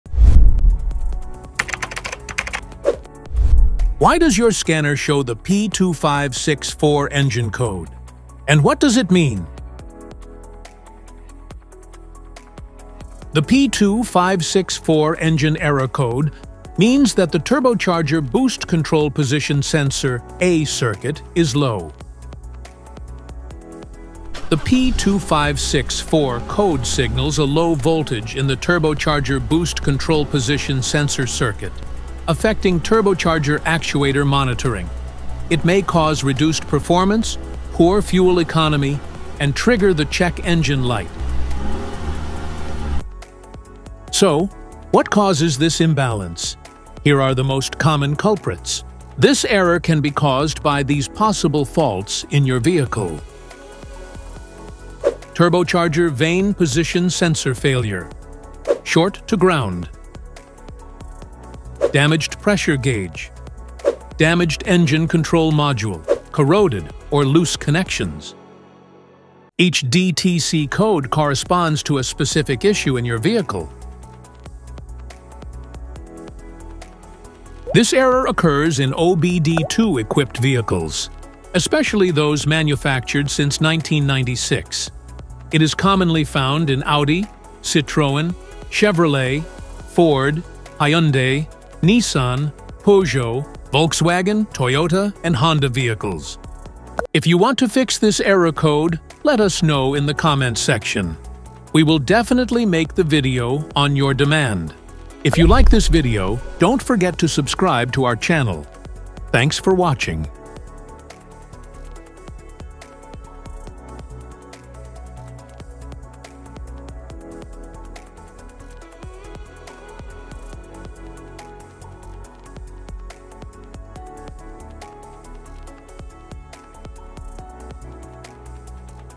What is P2564 : Engine Error Code Causes Explained
🚗 P2564 Engine Error Code: Turbocharger Boost Control Position Sensor Circuit Low
The P2564 code indicates that the Powertrain Control Module (PCM) has detected a low voltage signal from the turbocharger boost control position sensor circuit. This sensor monitors the turbocharger's position to ensure optimal boost pressure for engine performance.

📖 In This Video, You'll Learn:
✅ What the P2564 code means and its impact on your vehicle's performance.
✅ Symptoms such as poor acceleration, reduced power, and an illuminated Check Engine Light.
✅ Common causes like faulty sensors, wiring issues, or malfunctioning turbo actuators.
✅ Step-by-step solutions to diagnose and fix the error.

🚨 Risk Level:
⚠️ Moderate Priority
While you can still drive with this code, reduced engine performance and potential turbocharger damage can lead to costly repairs. Address it promptly.

🔧 Common Fixes for P2564:
Inspect Turbocharger Boost Control Position Sensor: Replace it if it’s defective.
Check Wiring and Connectors: Repair or replace damaged or corroded wiring in the sensor circuit.
Inspect the Turbocharger Actuator: Repair or replace the actuator if it’s stuck or malfunctioning.
Update or Reprogram the PCM: Ensure the PCM is functioning correctly and not sending false signals.
Check for Vacuum Leaks: Inspect and fix any vacuum line issues affecting turbo operation.
Test the Turbocharger: Ensure it’s functioning properly and not damaged internally.
👉 Pro Tip: Use a scan tool to monitor live data from the boost control position sensor to confirm its operation before replacing parts.